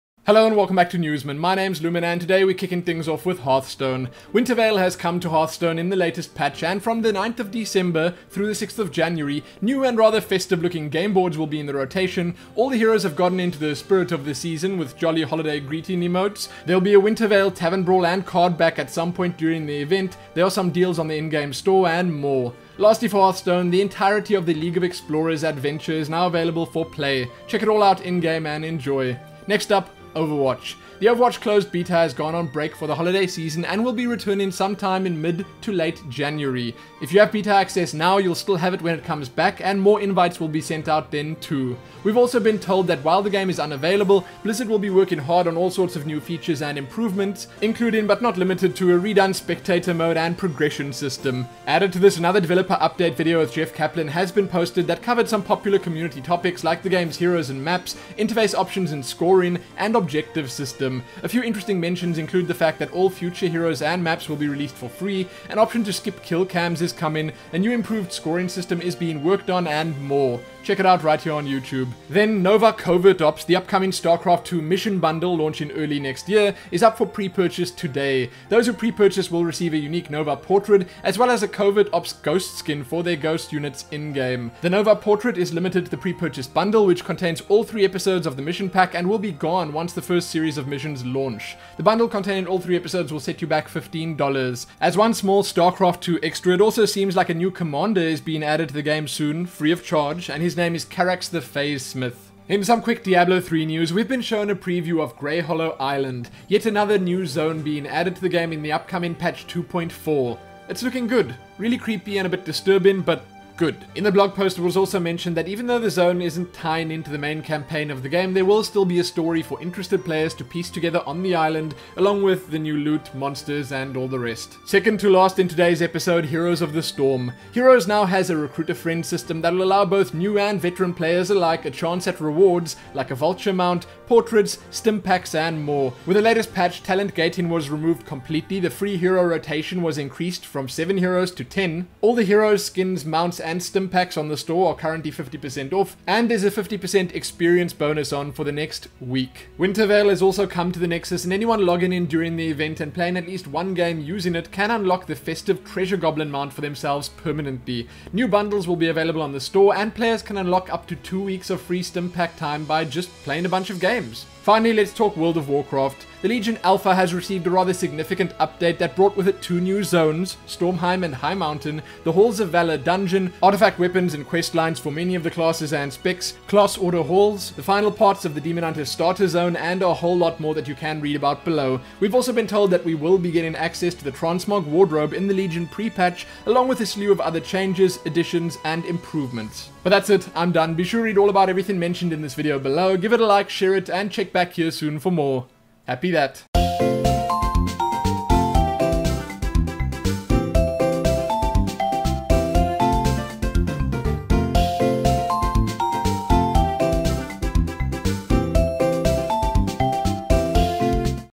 Winterveil in Hearthstone & Heroes, Greyhollow Island, Legion Updates & More! (Newsmin 13/12/15)
In this episode of Newsmin we're looking Winterveil in Hearthstone and Heroes, an Overwatch Developer Update and beta break, Nova Covert Ops for StarCraft II: Legacy of the Void, Diablo III Patch 2.4's Greyhollow Island, updates to the Legion beta and more!

► Hearthstone

Bundle Up for Winter Veil!

► Overwatch

► StarCraft II: Legacy of the Void

Pre-Purchase Nova Covert Ops™ Today!

►  Diablo III

► Lumin Links ◄